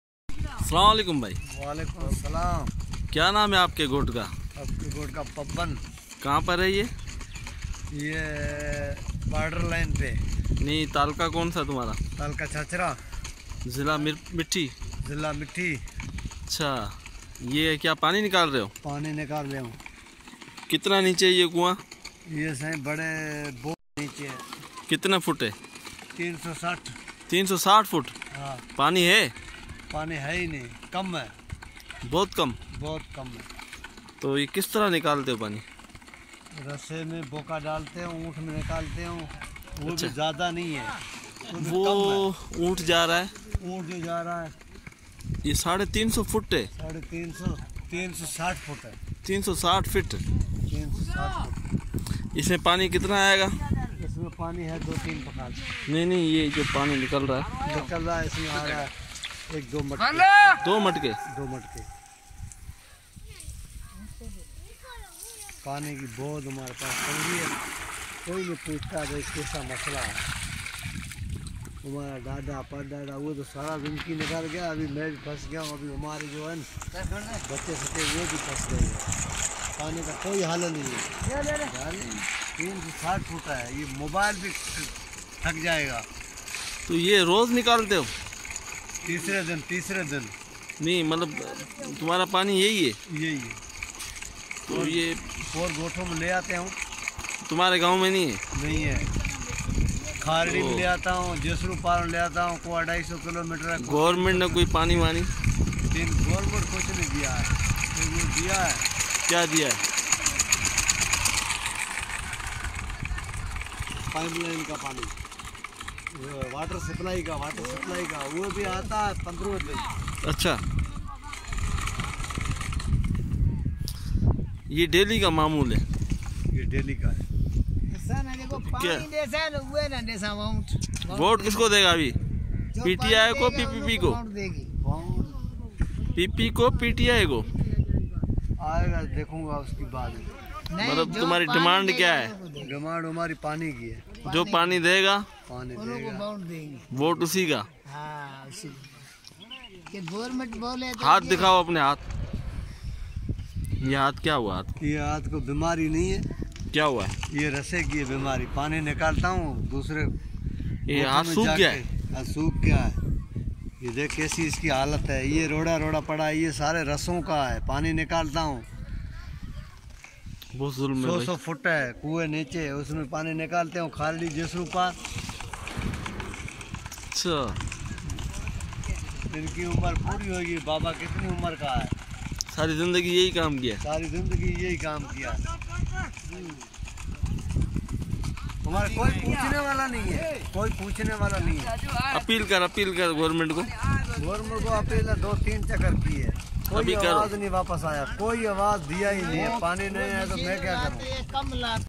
Desert Water Crisis |Documentary water issue in Tharparkar Villages
The Desert with no water: Imagine having to spend half your salary or more buying water, Ordinary families were the hardest hit